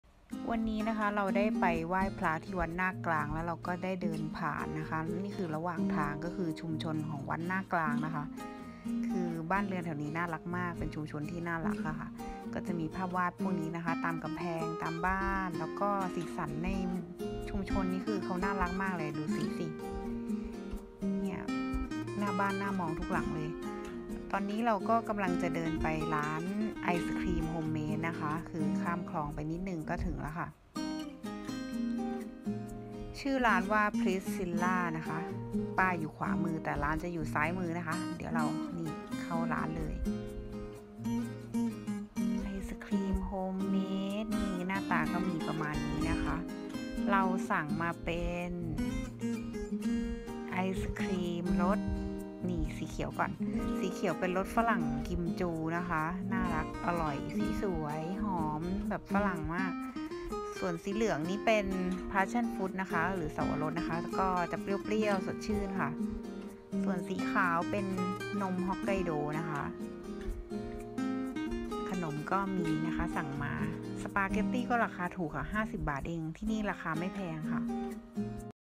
วัดนาคกลาง มีชุมชนน่ารักๆ และร้านไอศครีมโฮมเมด Prissilla
ทำธุระเสร็จ  ไหว้พระวัดนาคกลาง และได้เดินผ่านชุมชนน่ารักๆนี้ แวะทานไอศครีมอร่อย ชื่นใจ ราคาไม่แพง